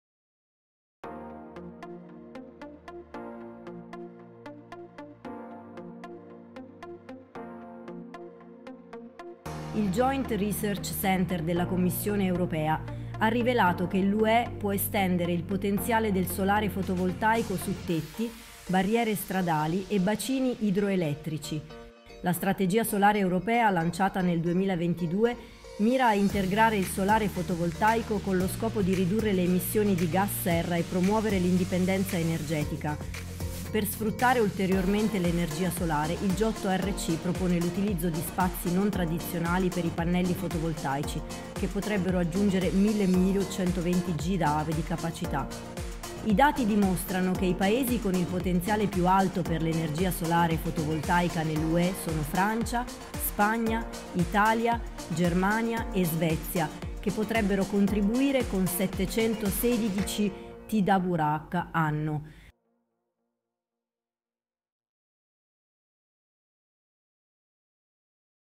Lo studio del JRC: è possibile ampliare il potenziale del fotovoltaico
Lo studio del JRC della Commissione europea afferma che la capacità di energia solare fotovoltaica dell'UE potrebbe aumentare di oltre 1TW grazie all'installazione su tetti, bacini idroelettrici e barriere stradali. Questo supererebbe di gran lunga l'obiettivo di 720 GW entro il 2030, aprendo nuove possibilità per l'energia solare in Europa.